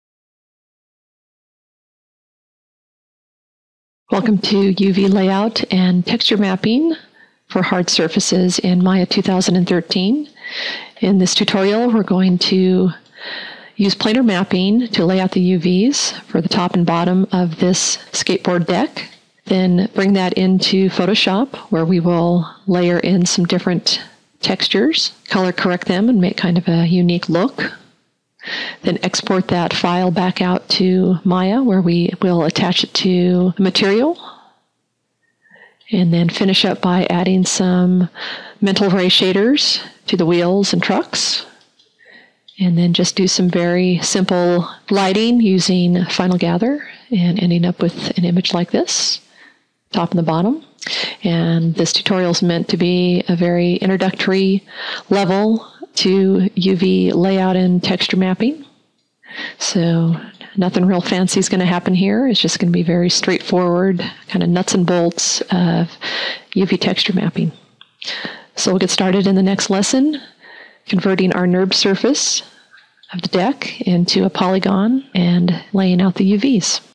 UV Texturing a Skateboard - Lesson 1 (Introduction)
In this short series, you'll use Maya 2013 to convert NURBS to Polys, then do a simple UV layout of a skateboard deck.  Graphics are added to the deck using Photoshop CS5, then attached in a basic shading network in Maya.  Additional shaders are added to the trucks and wheels, then a simple render is setup using Final Gather.
Level:  Beginner
Software:  Maya 2013, Photoshop CS5